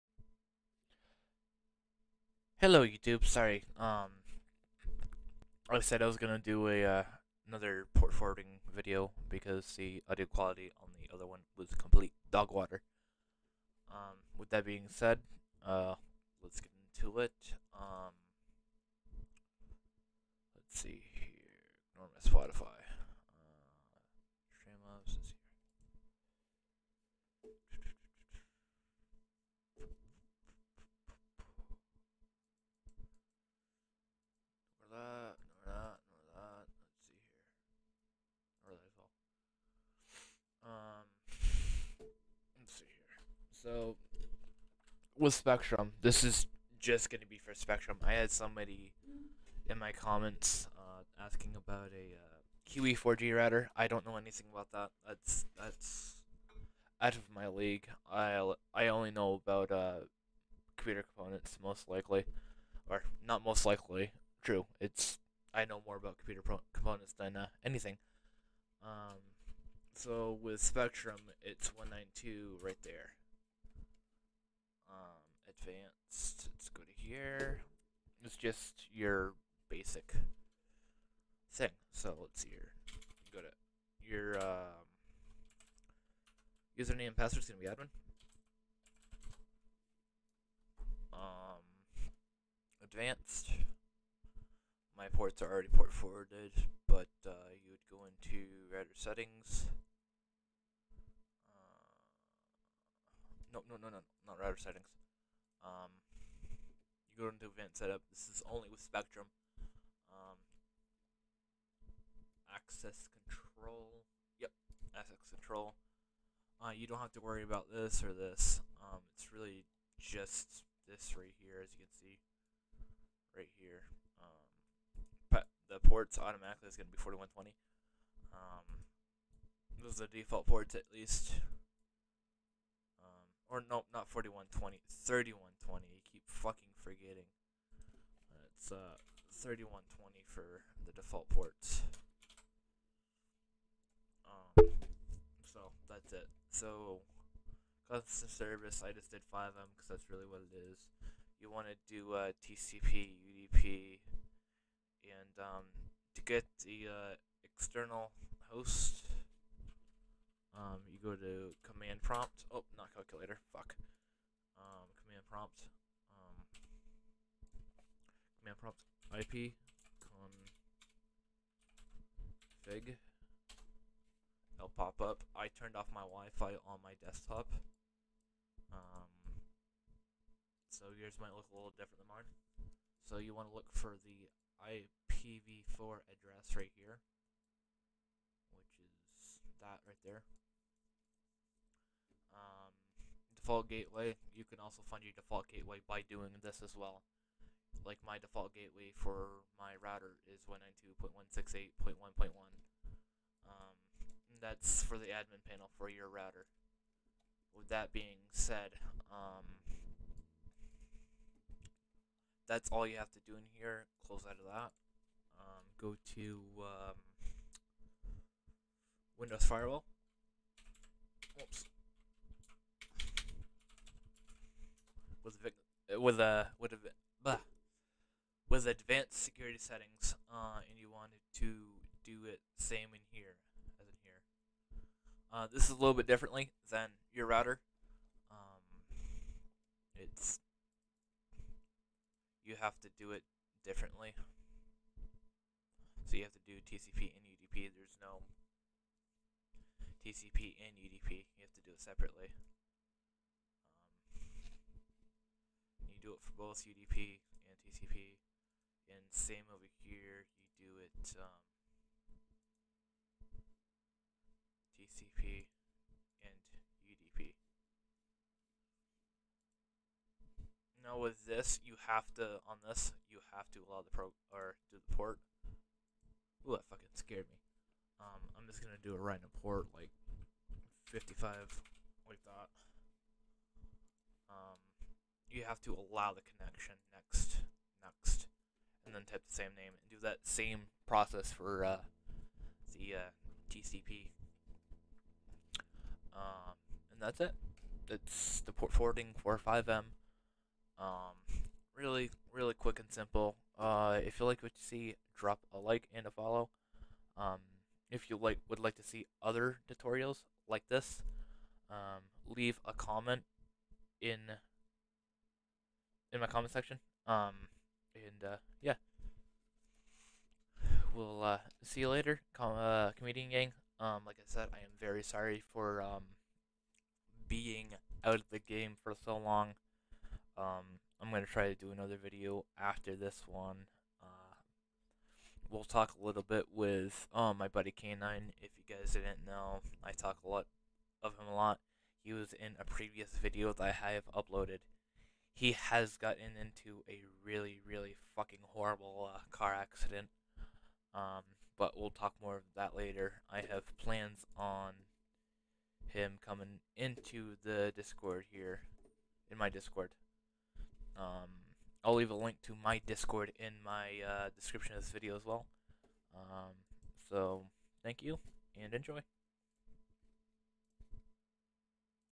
(Updated) how to port forward for fivem
hello and thank you for watching I have an updated port forwarding video for you i noticed on the last one that the audio quality was sh!t and I wanted to fix that so hear it is, please subscribe like and share and comment something you would like to see in the future